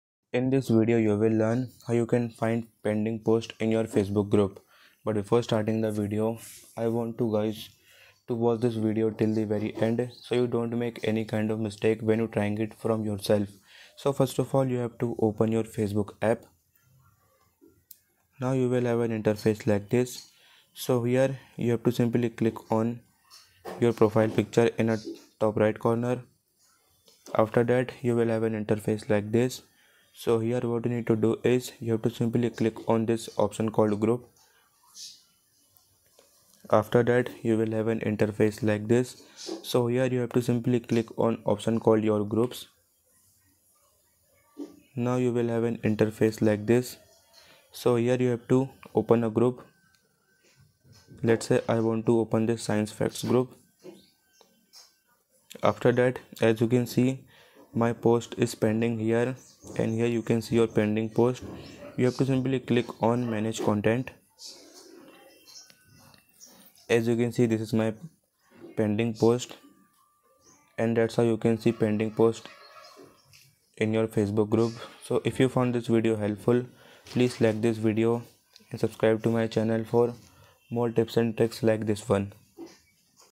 How To See Pending Posts on Facebook Group
Here's How To See Pending Posts on Facebook Group. Also I am going to show you about how to see my pending post on facebook group.